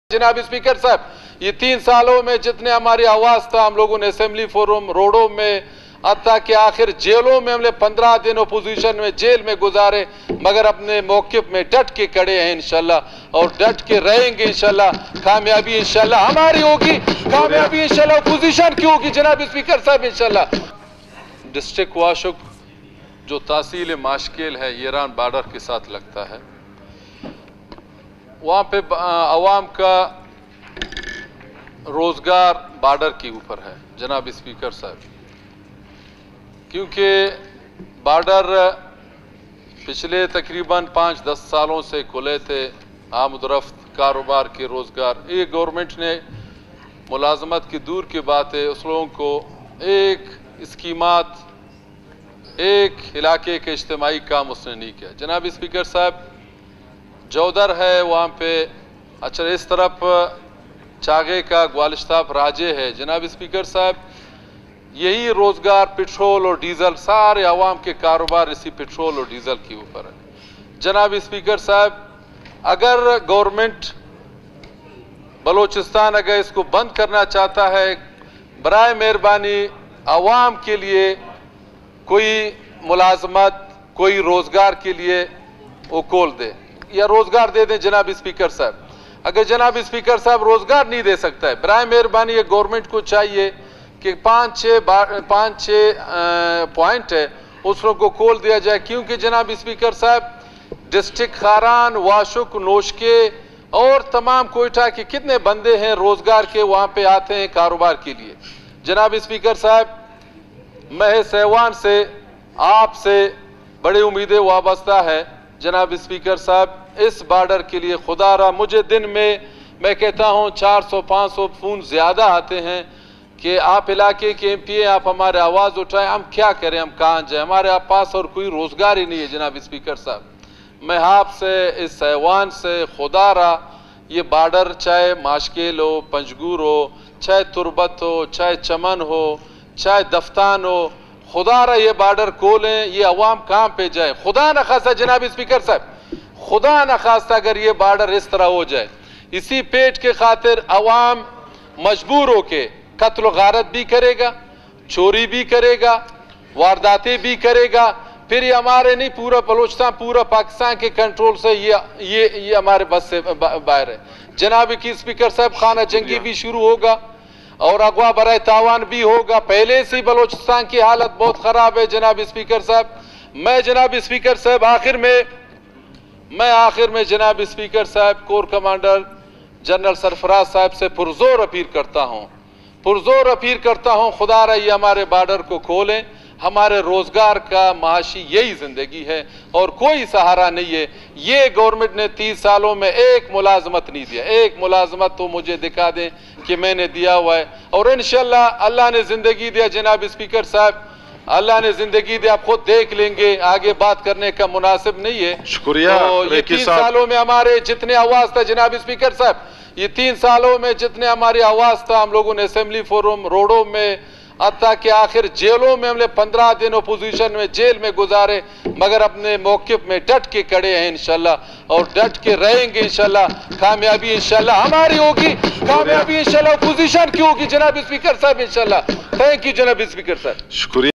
Haji Mir Zabid Ali Reki
ایم پی اے واشک حاجی میرزابد علی ریکی کورکمانڈر بلوچستان لیفٹیننٹ جنرل سرفراز آحمد سے درخواست ہے کے  تمام بارڈر کو کھول دیا جاۓ